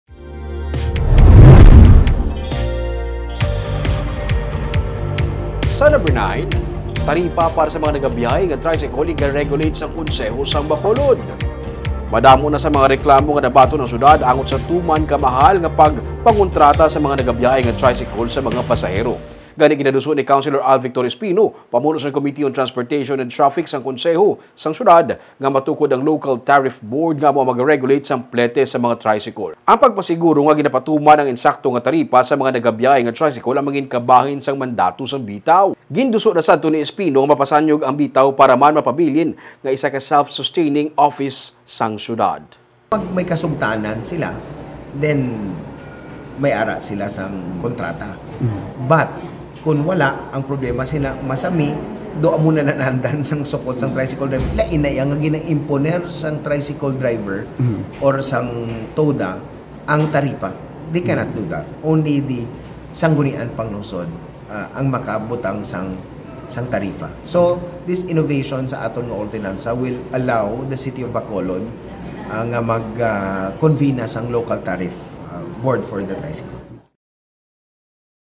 Suktanay sa tricycle
May mga nabaton nga reklamo ang konseho nga ka mahal sang sukatany sa plete sa tricycle gani plano nga i-regulate ang taripa